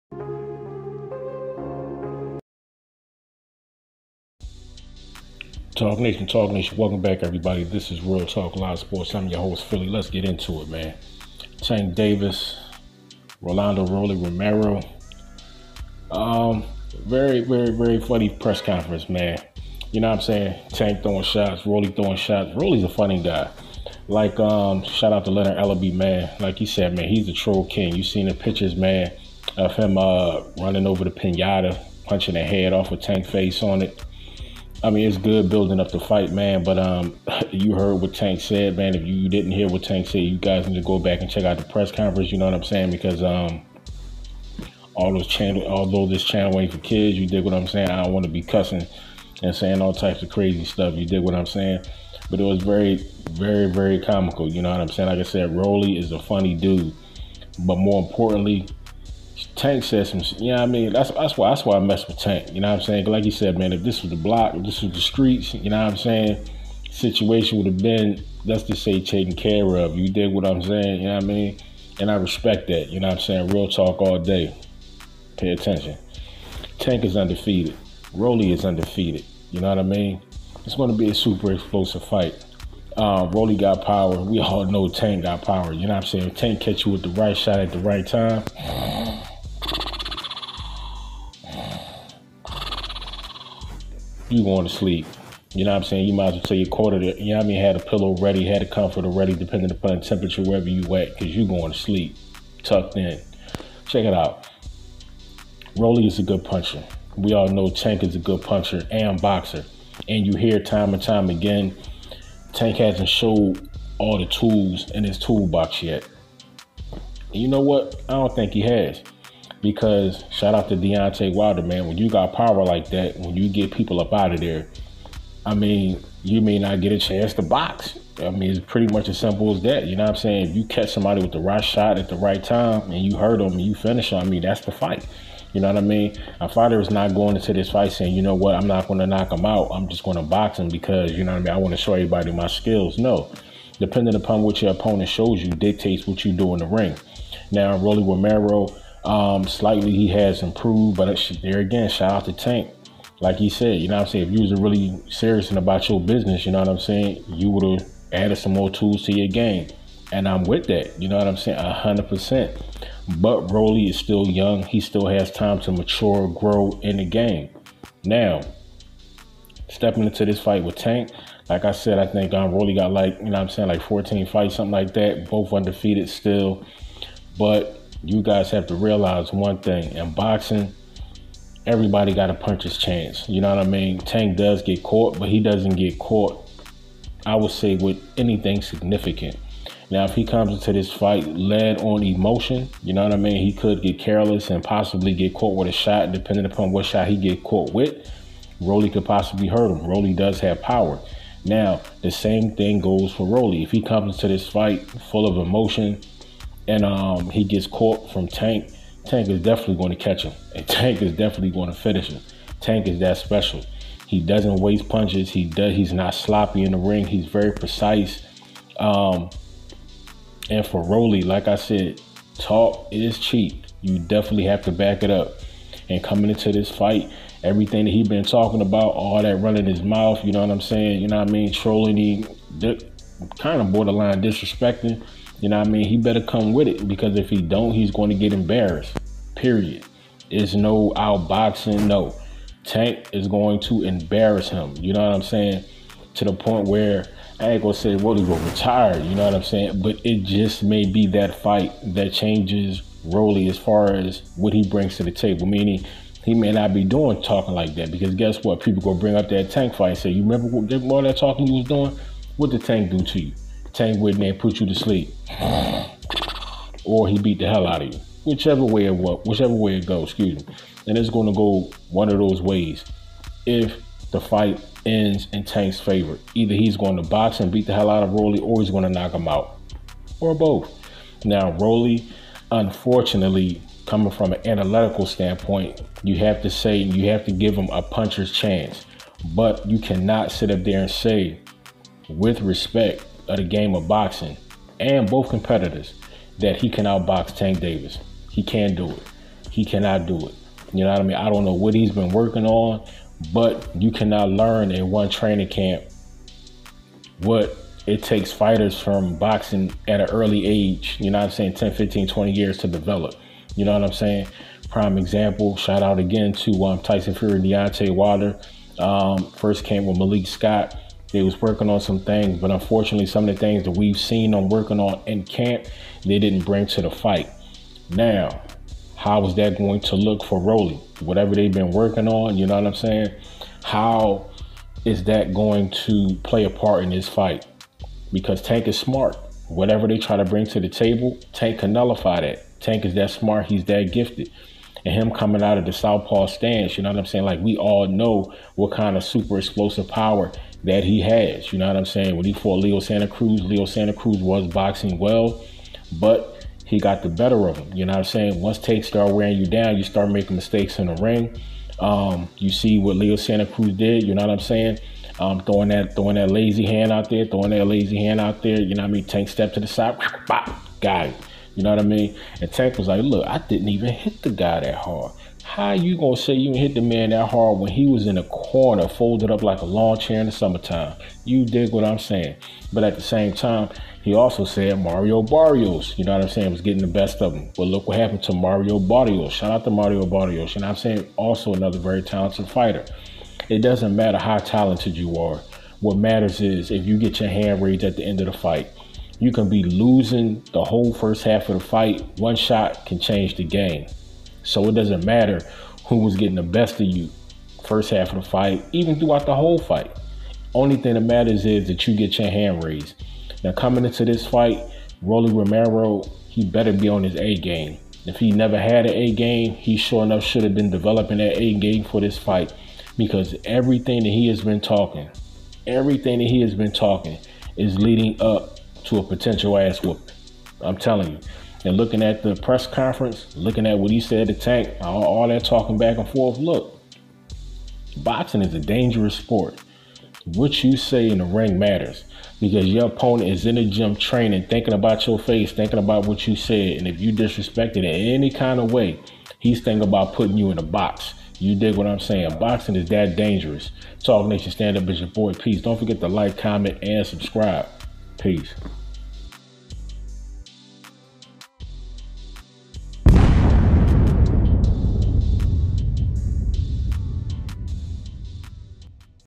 GERVONTA DAVIS: "ROLLY ROMERO YOU A CLOWN,YOU SCARED" | RealTalkLivesports
GERVONTA "TANK" DAVIS GETS HEATED DURING PRESSER AT ROLANDO "ROLLY" ROMERO AHEAD OF DECEMBER 5TH SHOWDOWN LIVE ON SHOWTIME PPV .